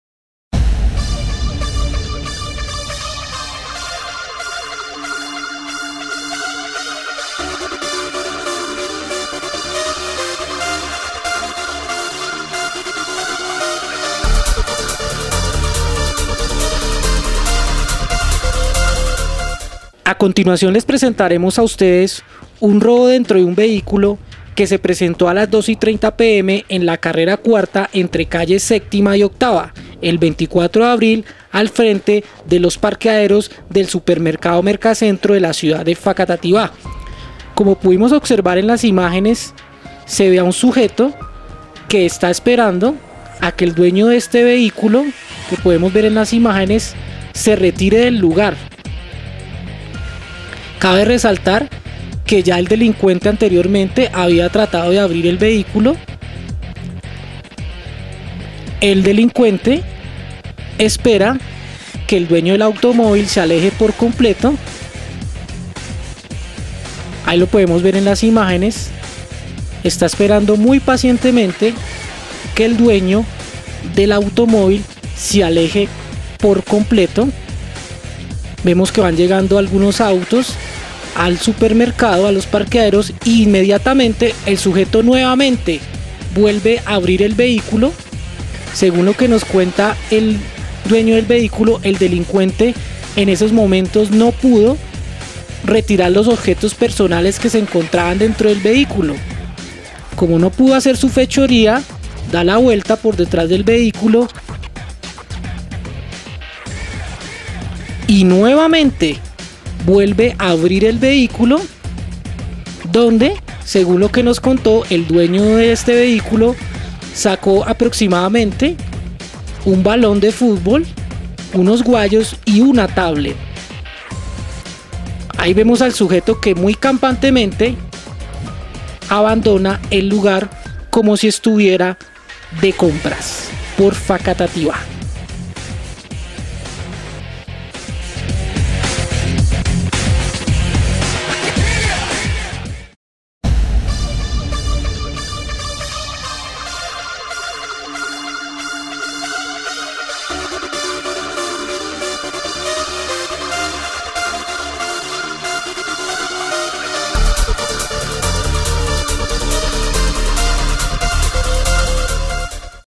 ROBO DENTRO DE UN VEHÍCULO EN FACATATIVÁ
Por no dejar su vehículo en una parqueadero a este conductor le saquearon su automóvil, donde le robaron algunos objetos personales por un valor aproximado de un millón de pesos, tenga en cuenta esto usted pude ser la próxima victima .